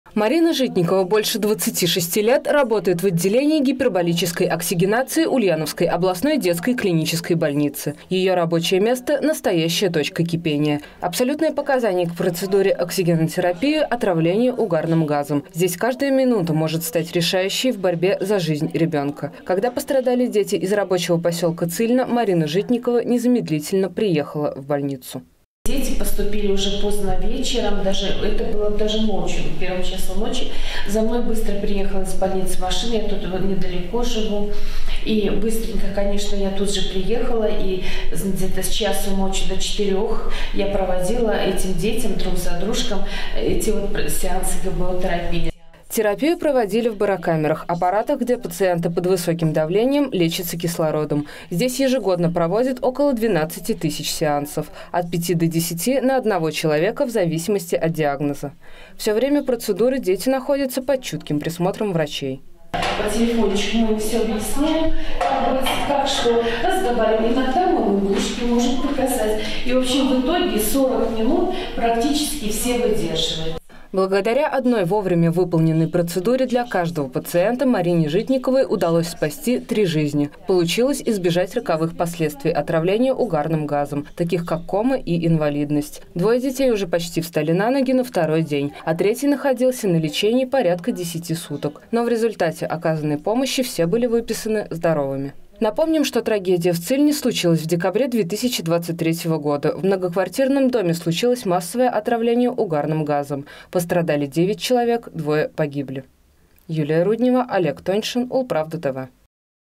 До последнего за жизнь каждого ребёнка. Как работает врач, которая спасла детей из Цильны
Врачей, спасающих жизни, по праву можно назвать героями. Особенно тяжело приходится тем, кто работает с детьми, находящихся в тяжёлом состоянии. Например, ульяновский педиатр Марина Житникова поставила на ноги трёх детей, которые отравились угарным газом в Цильне.